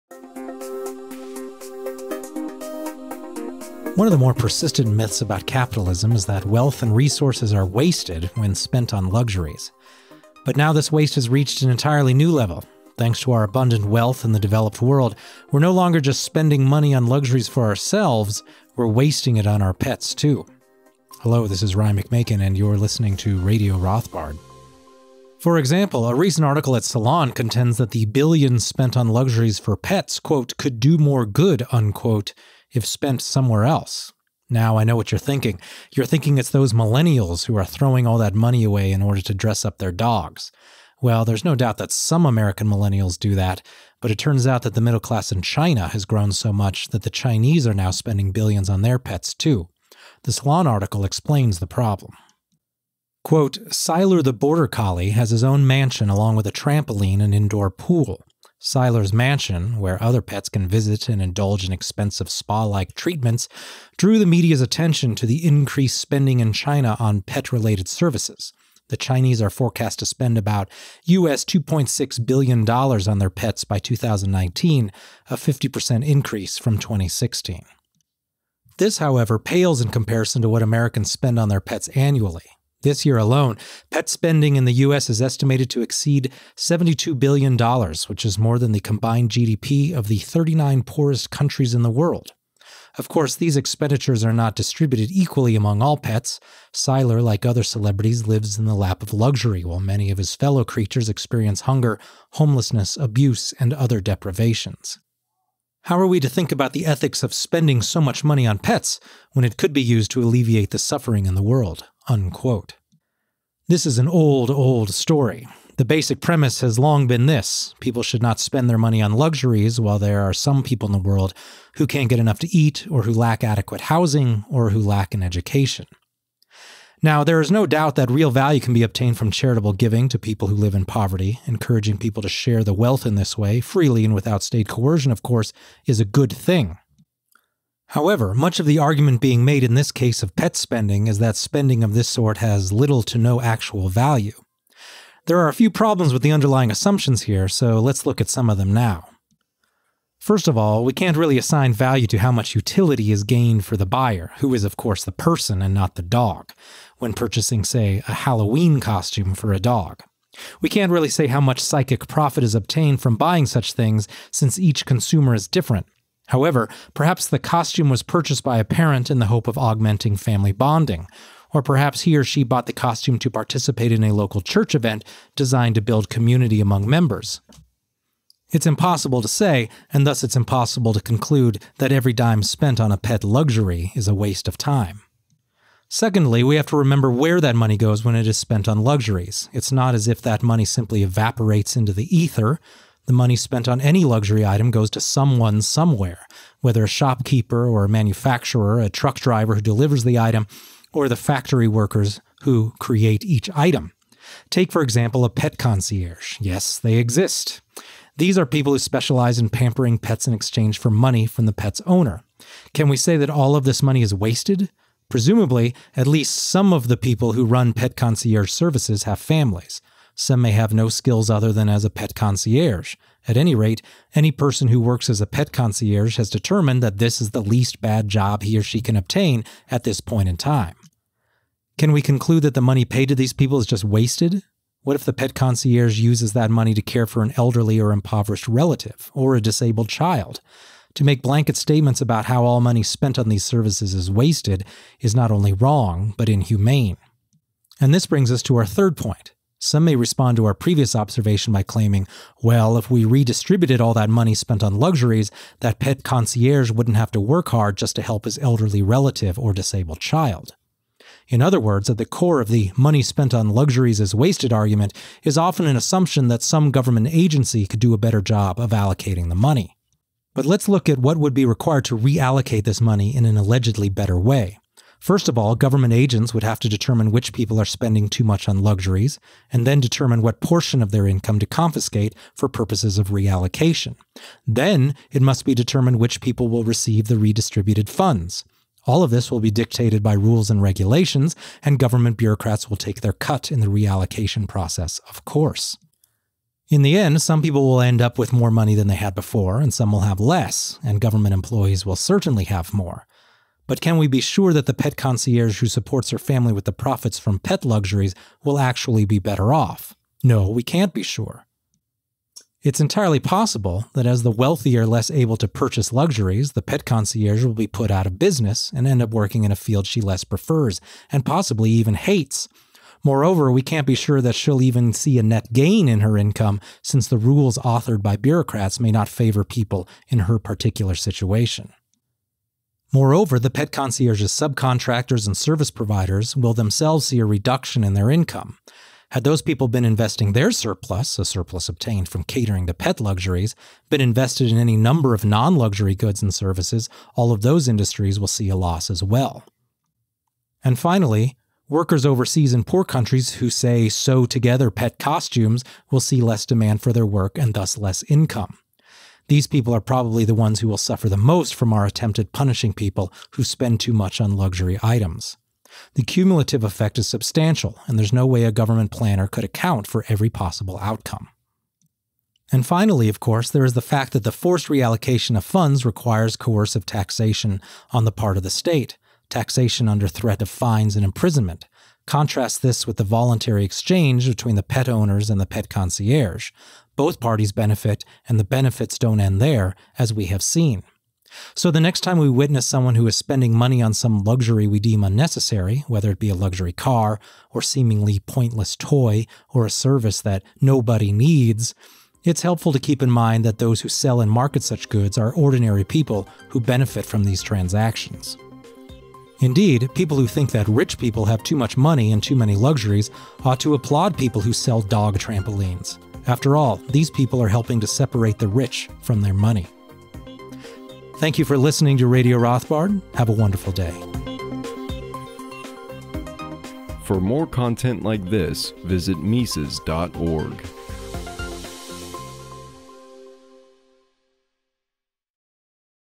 It's OK To Buy Your Pet a Halloween Costume (and Other Luxuries)
Money spent on "luxuries" goes to support ordinary people who use profits from luxury goods to make a living and support their families.

Original article: Mises.org/wire/its-ok-buy-your-pet-halloween-costume-and-other-luxuries

Ryan McMaken (Mises.org/McMaken) is the editor of Mises Wire and The Austrian